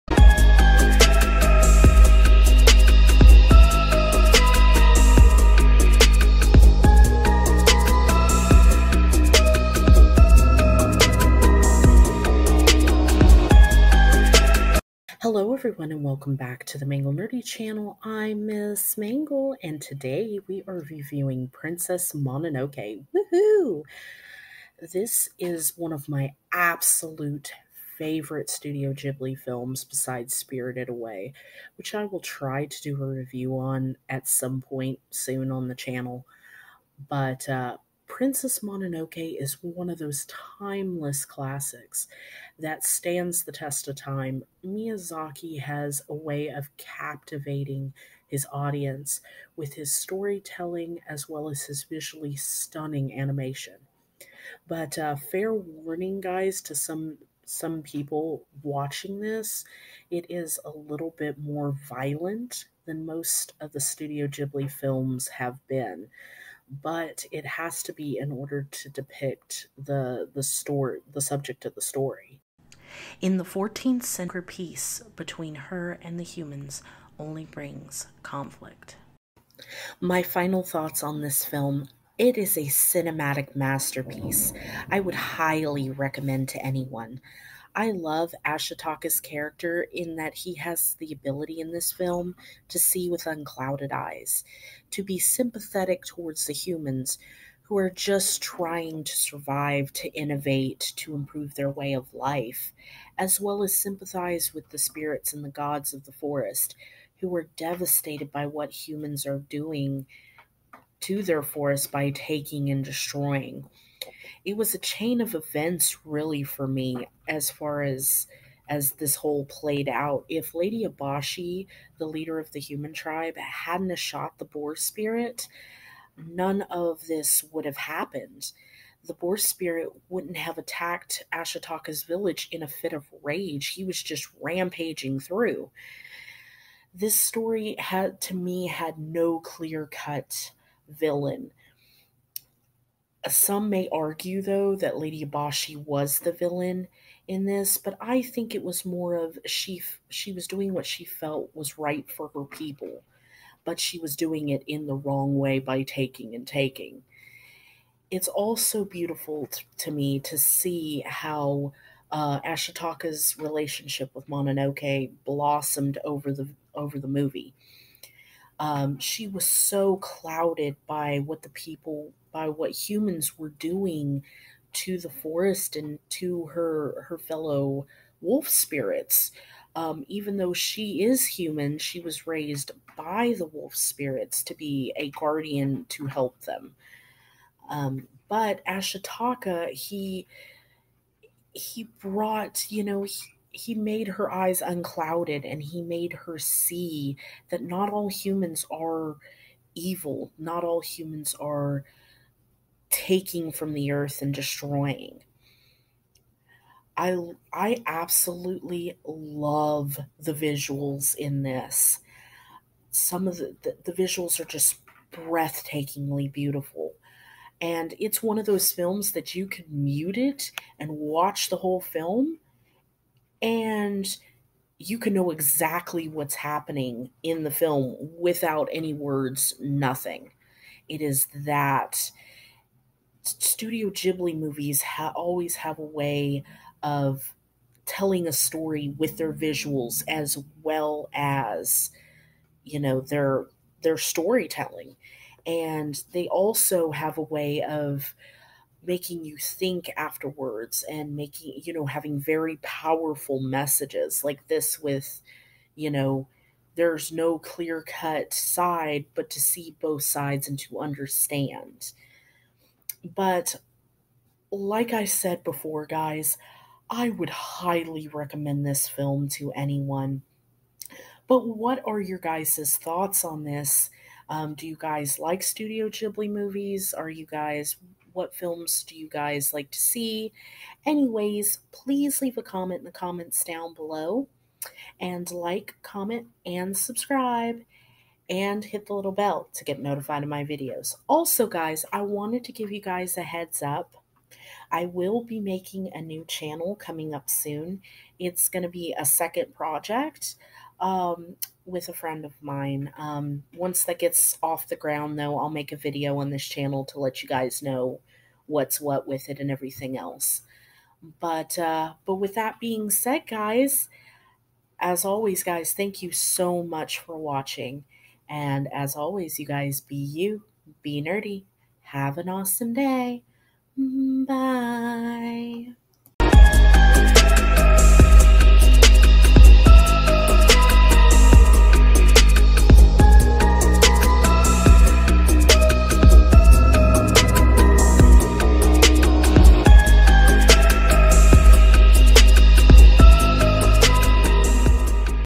Mangle reviews :Princess mononoke (1997)
Hello everyone i hope yall enjoy my review of Princess Mononoke. This film is a must watch film I highly recommend for everyone. As always Be you Be Nerdy Have an awesome day!!
P.S: this is a re upload i deleted it by accident sorry for the inconvenience